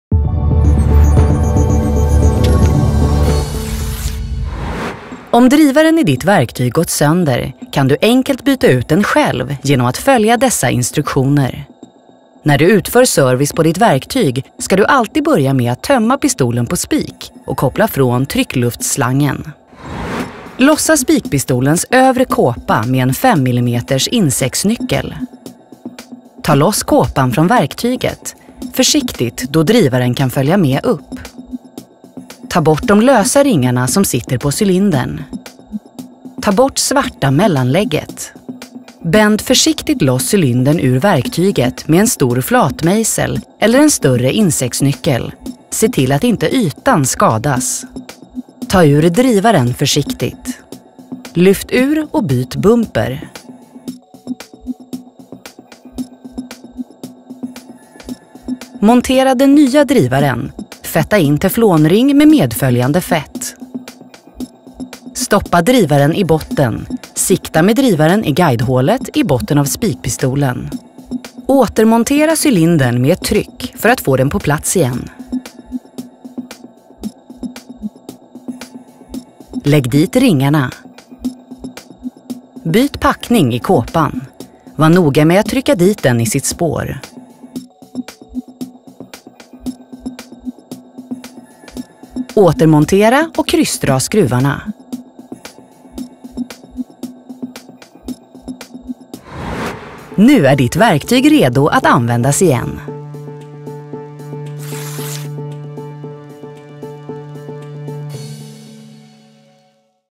Drivarbyte Spikpistol - Instruktionsfilm | ESSVE
Spikpistoler och kompressorer kräver kontinuerlig service för maximal funktion och lång livslängd. Mycket av servicen som behövs kan du enkelt utföra helt själv. Om drivaren i ditt spikverktyg gått sönder kan du enkelt byta ut den själv genom att följa instruktionerna i denna film.

Detta är en av ESSVEs instruktionsfilmer som guidar dig igenom de moment av spikpistol- och kompressorservice som du enkelt kan utföra på egen hand.

Att utföra drivarbyte på spikpistolen är enkelt!